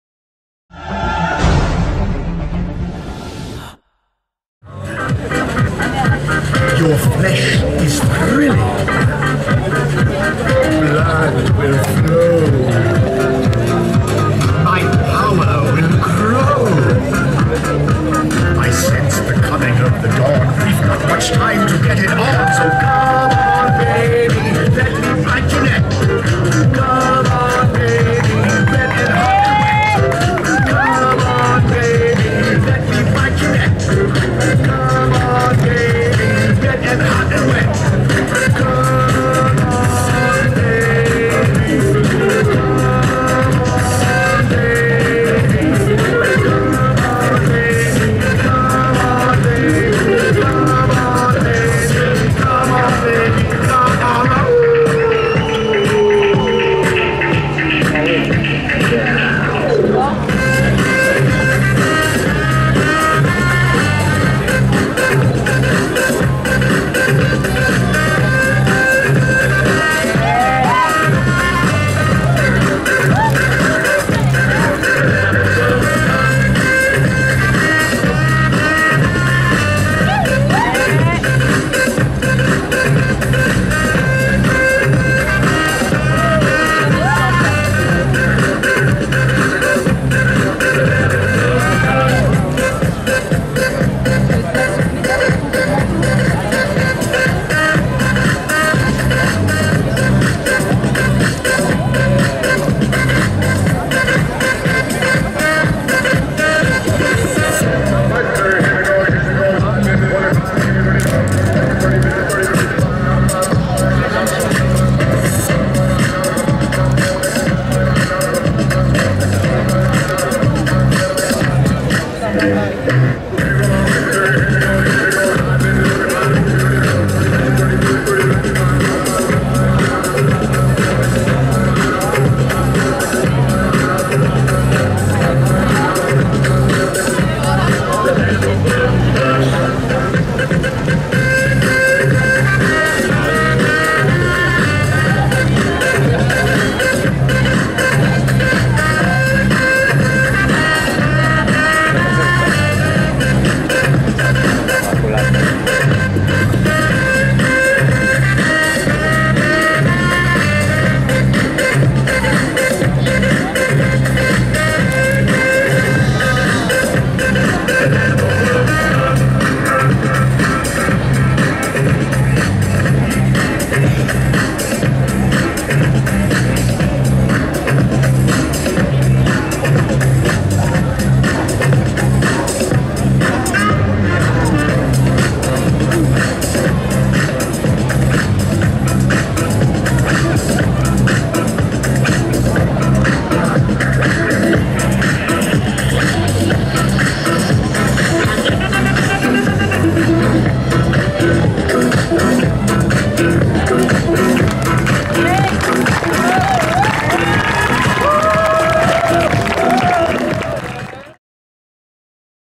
► Zombie Walk (Paris 2015) - Performance Poï AD+ (Nymeria Firekiss)
Pour cette 8ème édition de l'invasion des morts on ne peut plus vivants dans la capitale, la dynamique troupe de jongleurs et cracheurs de feu Poï AD+ nous a offert un joli show enflammé Place de la République ! (captation vidéo du samedi 3 octobre 2015)